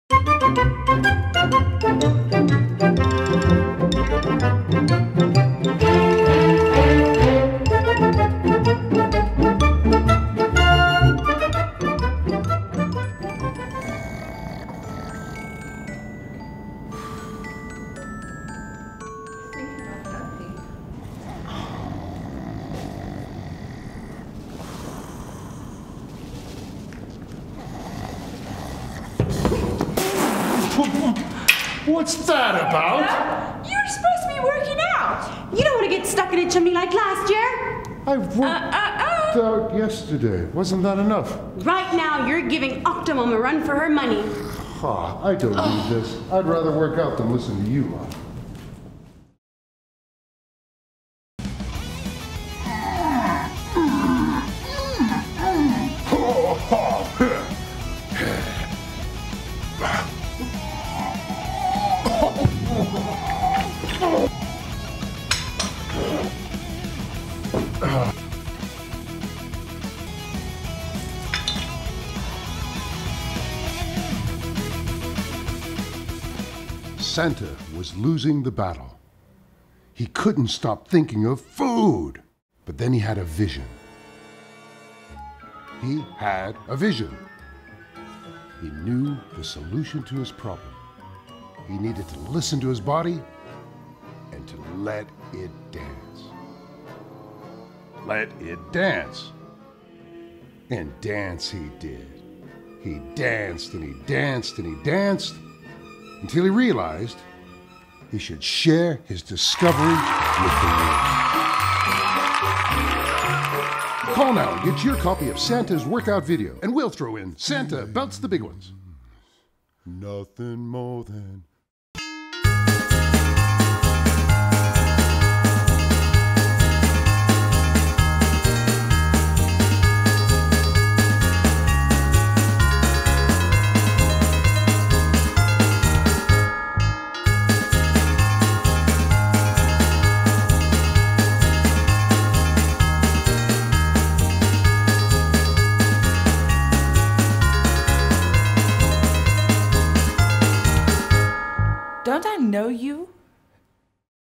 Santa Regular Guy - Santa works out
Santa's a regular guy, who needs some time in the gym.